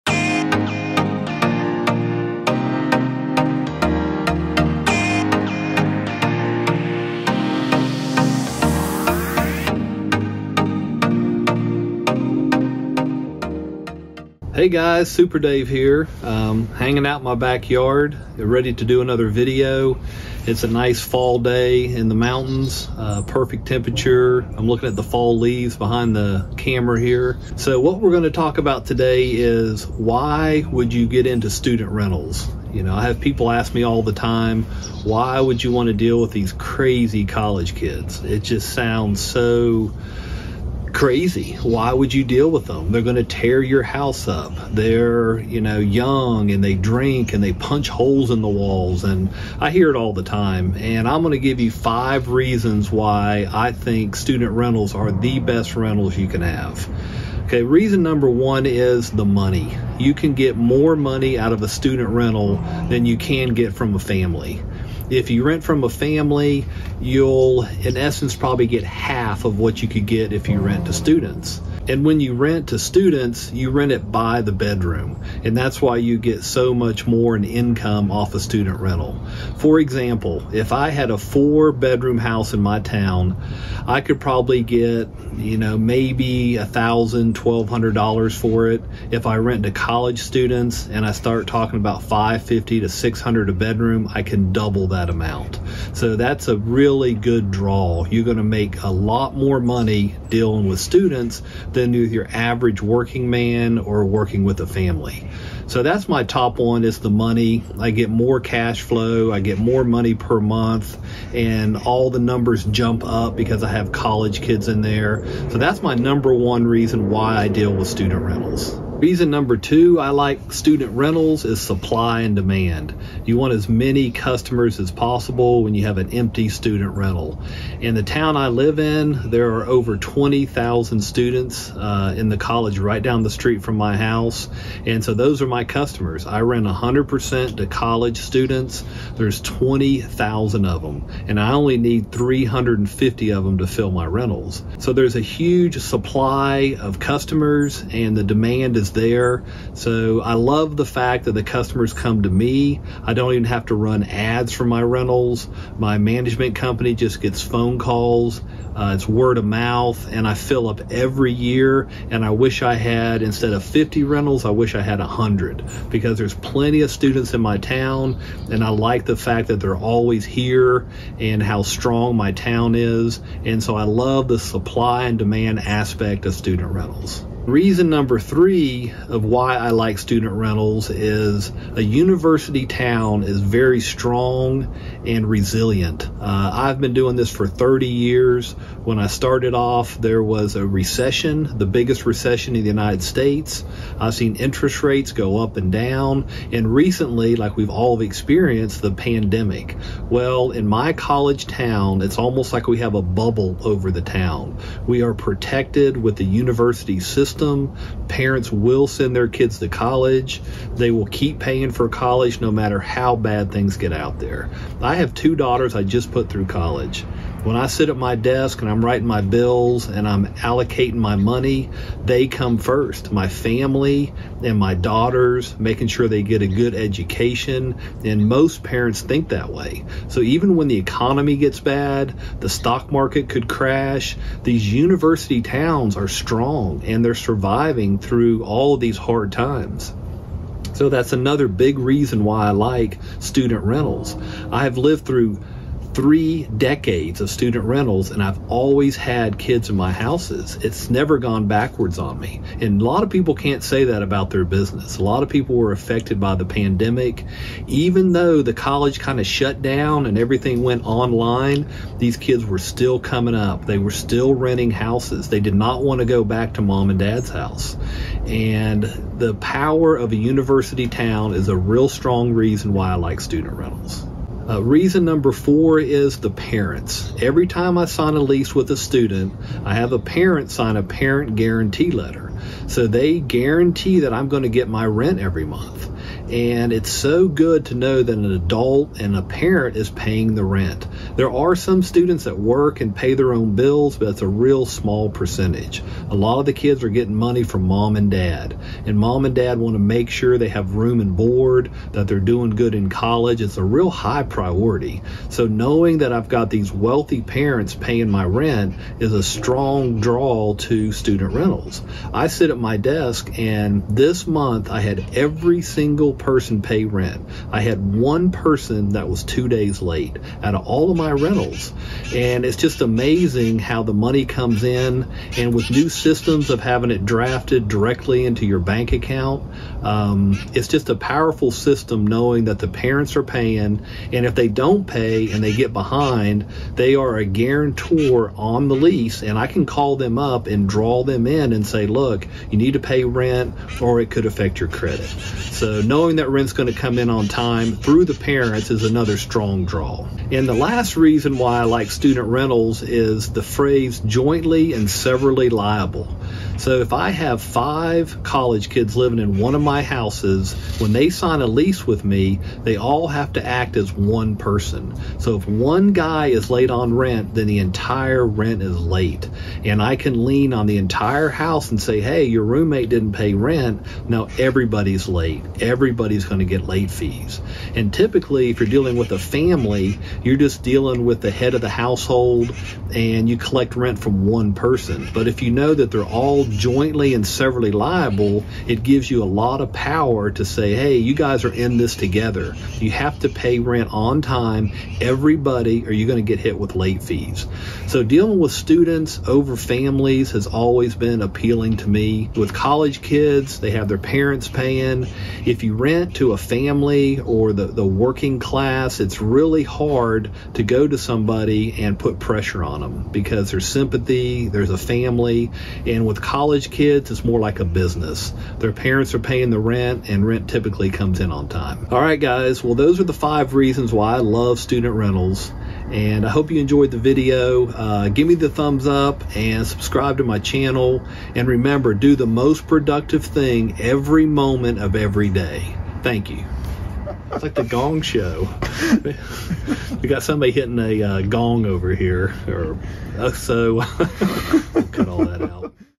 Why Does SuperDave LOVE Student Rentals?? [5 Reasons]
Hey everybody! Today I'm talking about 5 reasons I love student rentals, and why I think more people should look into renting to college students! From more cashflow to severally and jointly liable, student rentals provide a unique and lucrative avenue to invest in real estate!